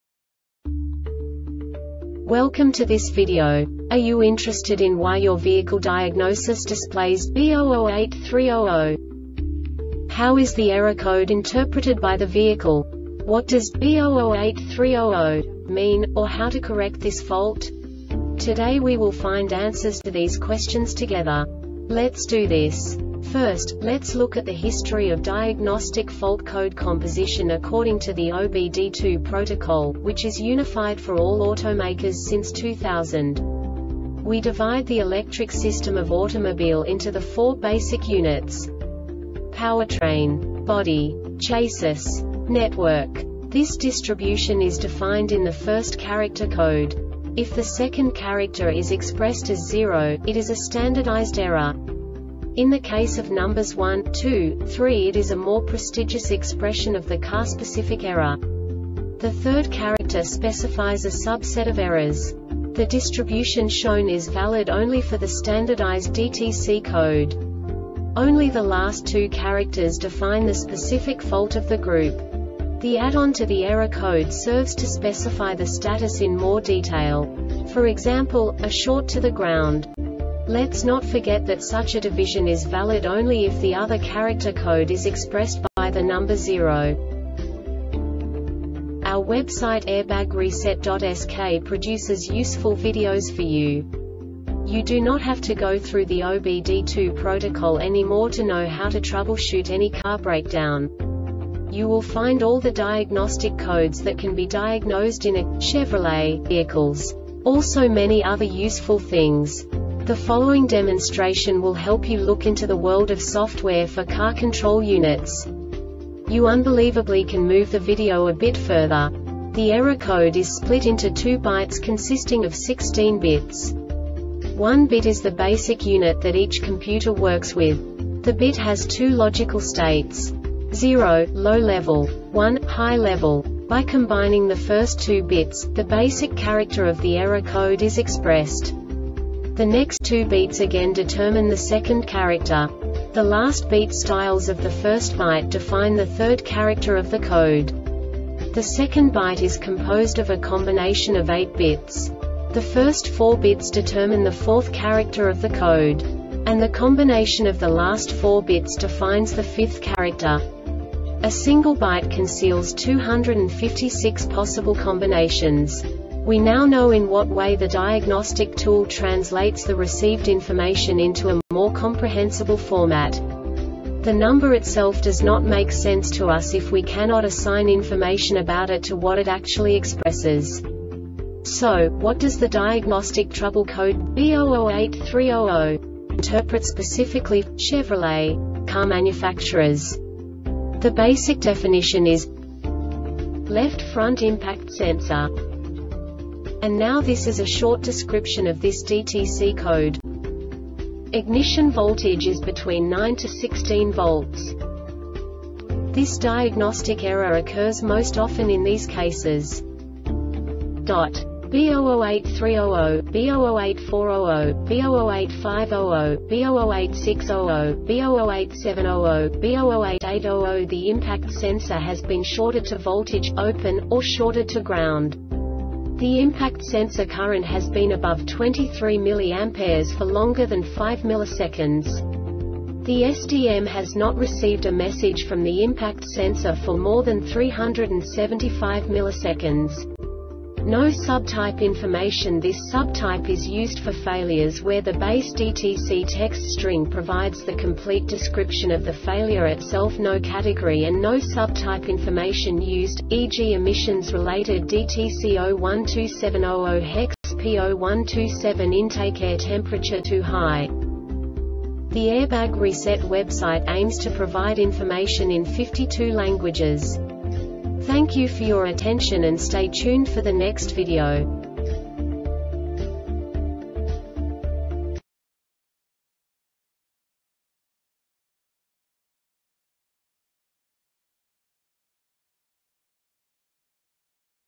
DTC Chevrolet B0083 00 Short Explanation
Contents:
0:21 Basic DTC analysis according to OBD2 protocol standard.
1:48 Insight into programming
2:58 A diagnostically understandable description of the Diagnostic Trouble Code
3:05 Basic error definition

Make:
Chevrolet
Code:
B0083 00
Definition:
Left Front Impact Sensor
Description:
Ignition voltage is between 9-16 volts.
Cause:
The impact sensor has been shorted to voltage, open, or shorted to ground.
The impact sensor current has been above 23 mA for longer than 5 milliseconds.
The SDM has not received a message from the impact sensor for more than 375 milliseconds.